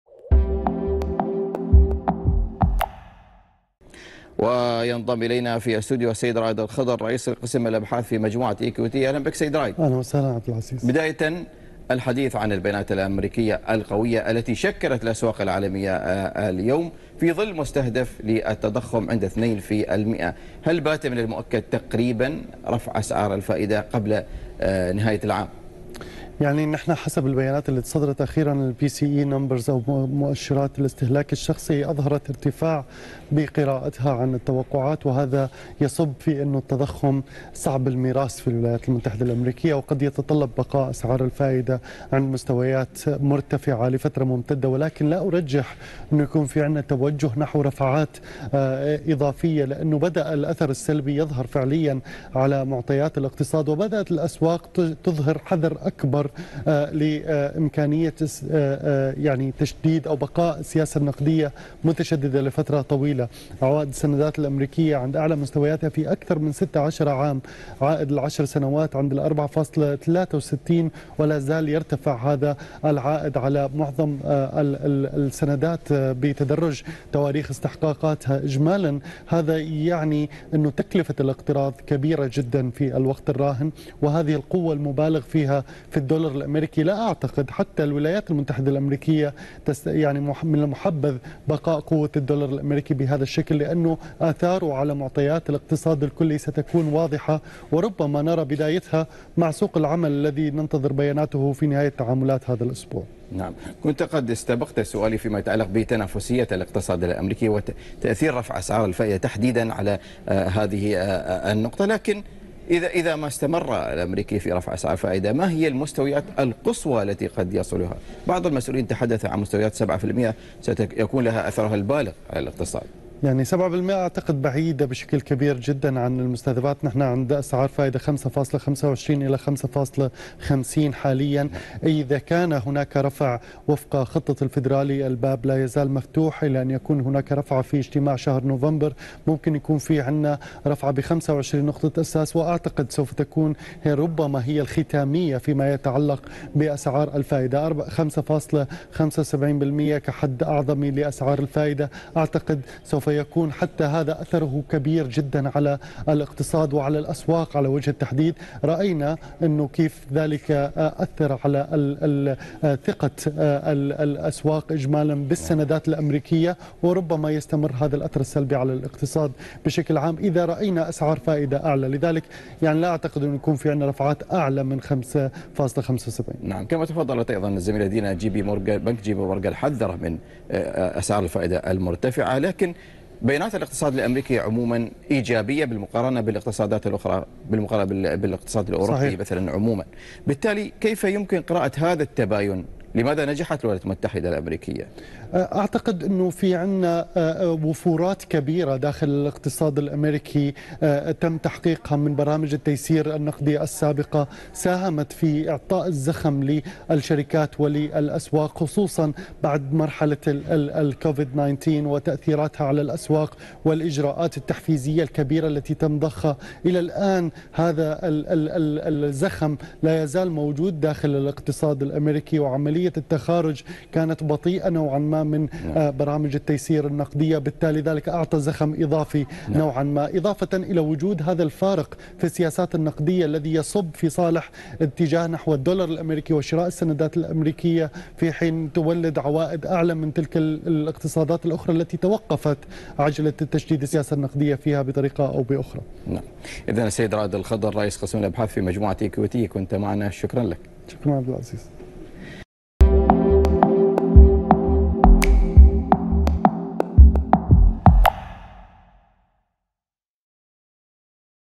هل سيواصل الفيدرالي الأمريكي الاحتفاظ بأسعار الفائدة مرتفعة ‏لأطول فترة ممكنة؟
عززت بيانات اقتصادية قوية في الولايات المتحدة وتعليقات متشددة ‏من مسؤولي الاحتياطي الفيدرالي من احتمالات رفع أسعار الفائدة ‏الأمريكية بنحو 25 نقطة أساس قبل نهاية هذا العام.
عوائد السندات الأمريكية عند أعلى مستوى لها في 16 عام.